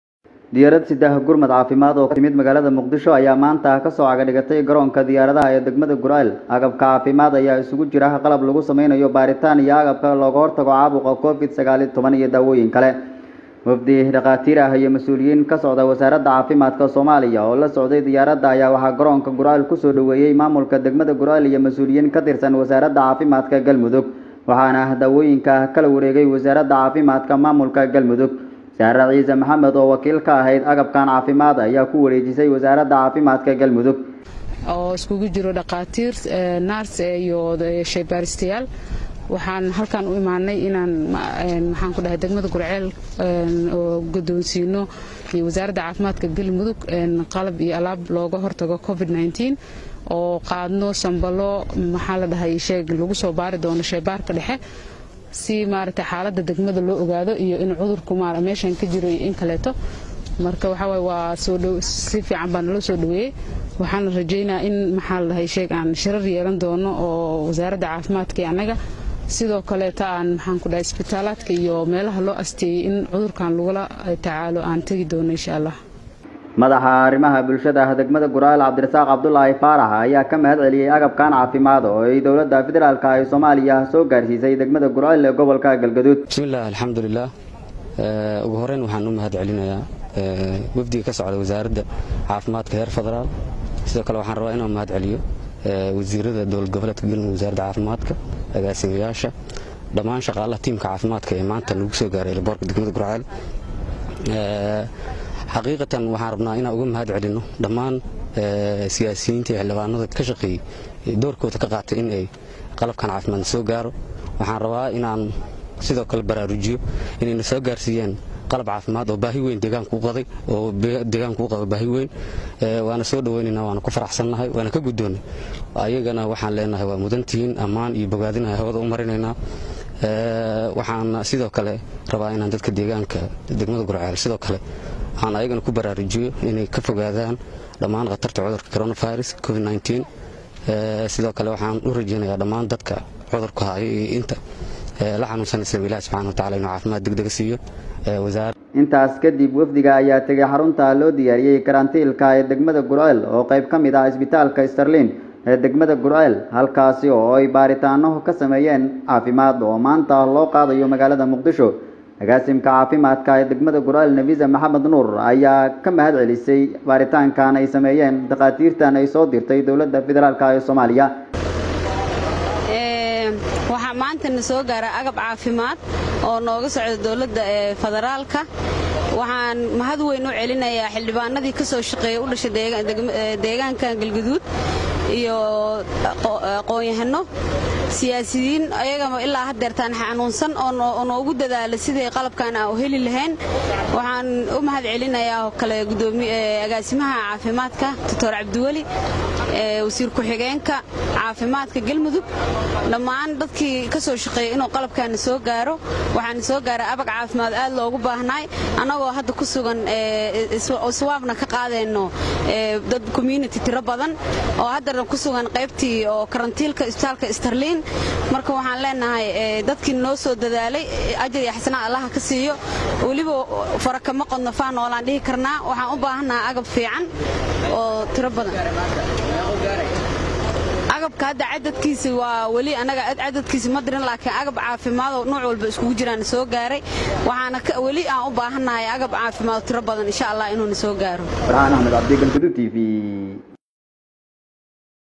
Dowladda Soomaliya ayaa Guriceel soo gaarsiisay agab Caafimaad
Dowladda Soomaliya ayaa maanta Guriceel Soo gaarsiisay agab Caafimaad